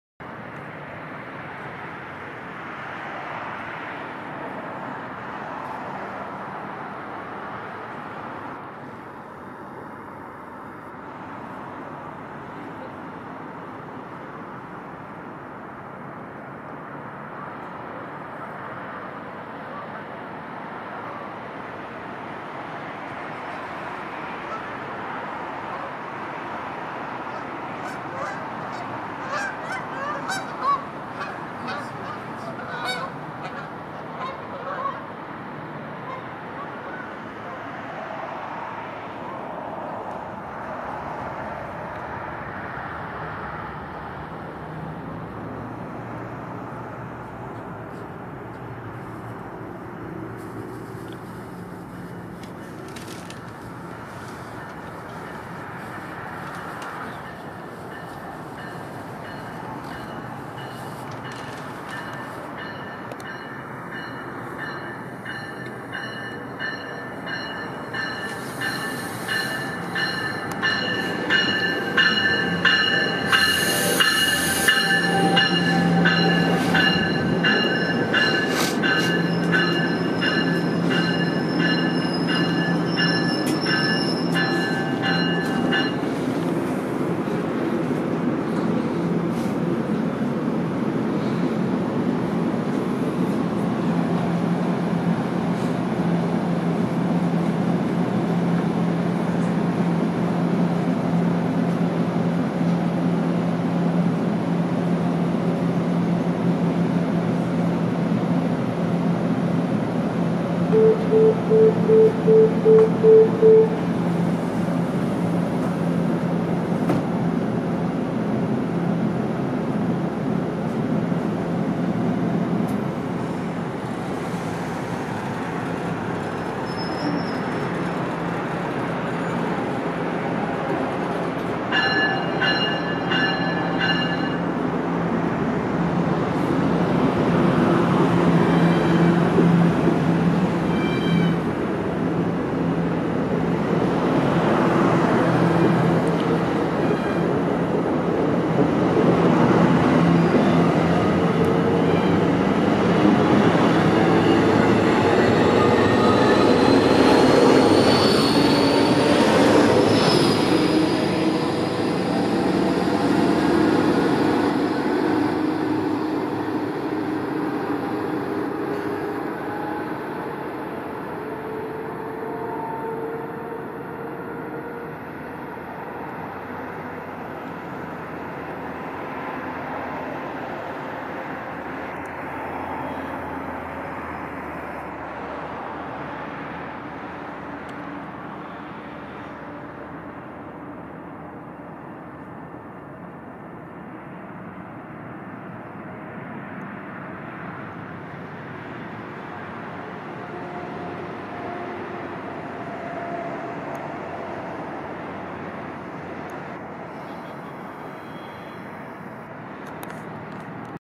Siemens Charger doubleheader on MARC 495
While still at Halethorpe, we see MARC Train 495, the Penn Line local to Washington Union Station.  495 consists of MARC IV cab car 8057, three additional MARC IV Multilevel coaches, a MARC II coach, and Siemens Charger SC-44's 86 and 81 on the rear.  Enjoy the view of the doubleheader train.  If you have any questions about the video, feel free to message me.  Comments are welcome.  The video was taken on February 23rd, 2020.